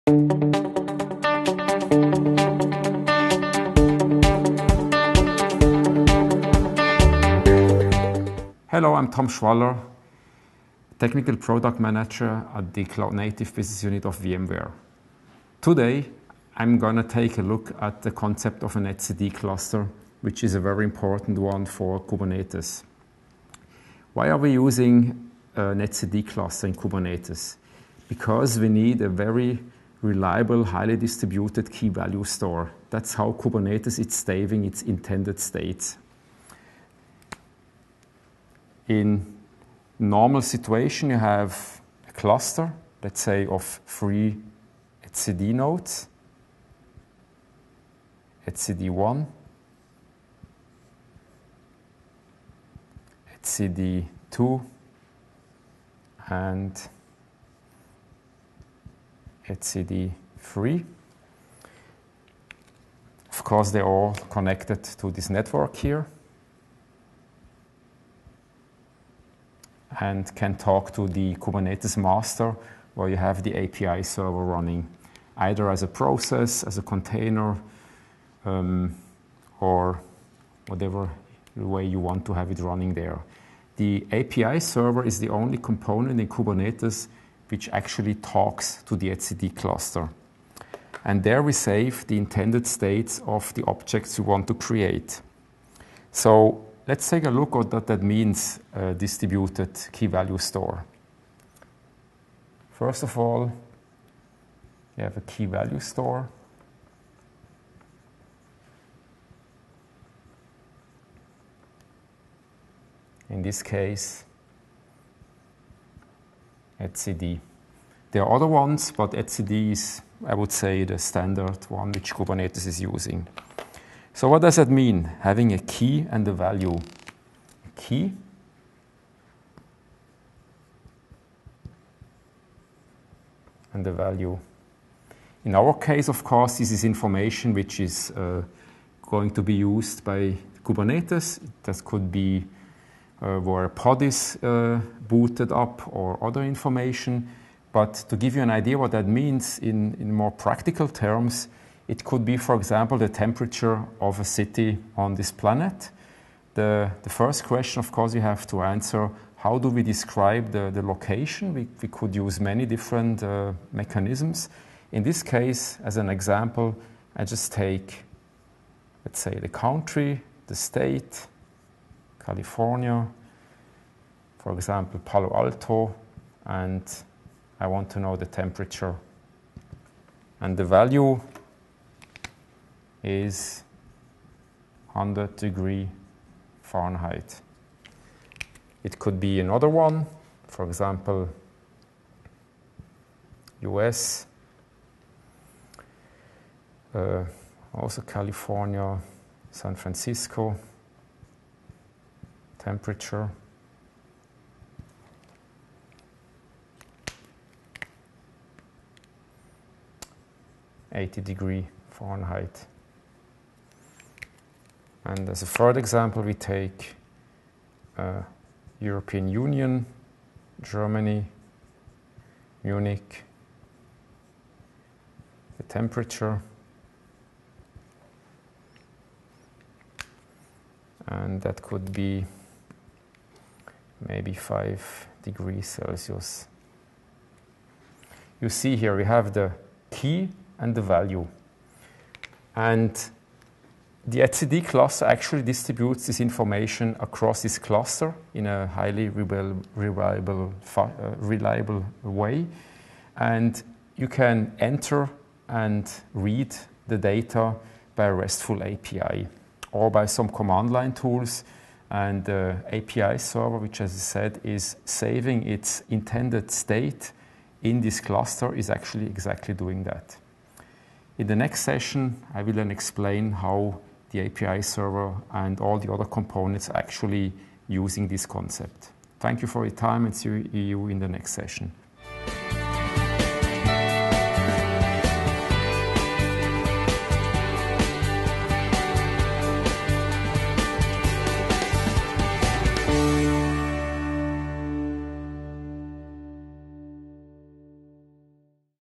Understanding Kubernetes, Part 1: etcd clusters and key/value stores
In this 3-part video series, Technical Product Manager Tom Schwaller goes through the components that make up a Kubernetes cluster, a key component of Pivotal Container Service (PKS).

In this first video, Tom describes the etcd clustering architecture of Kubernetes, and its key/value store model.